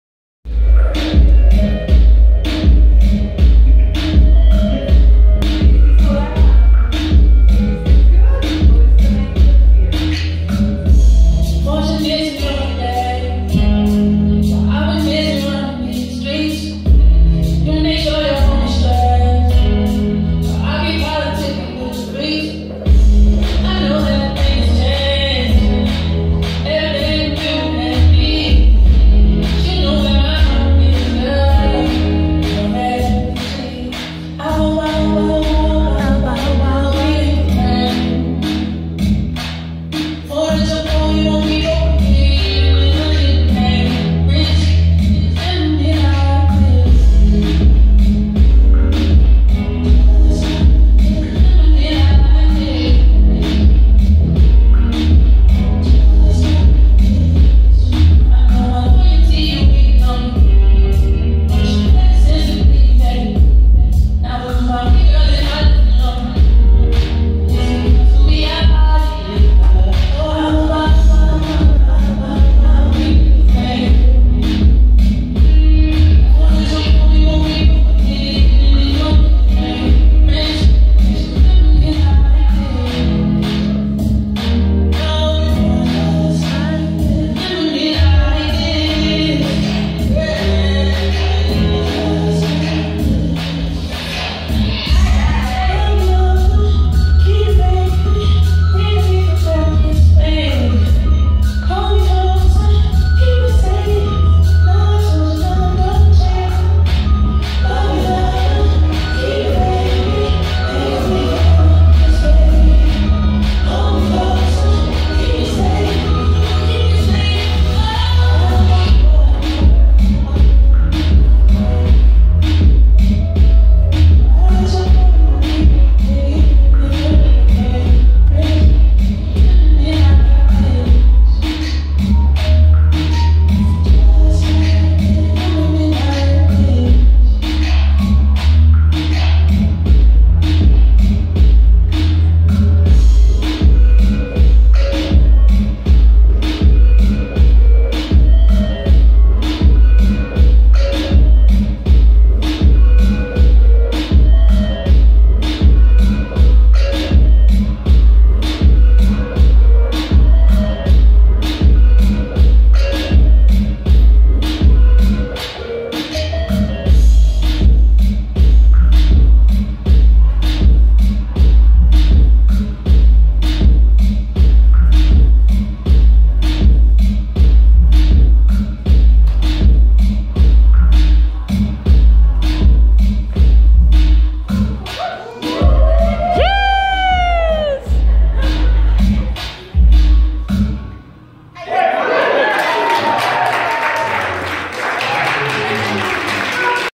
Tarah + Pilou - Urban Kizomba Advanced @Urban Legends - Toronto 2018
Demo at Urban Legends II in Toronto ! Advanced Leading and Following Workshop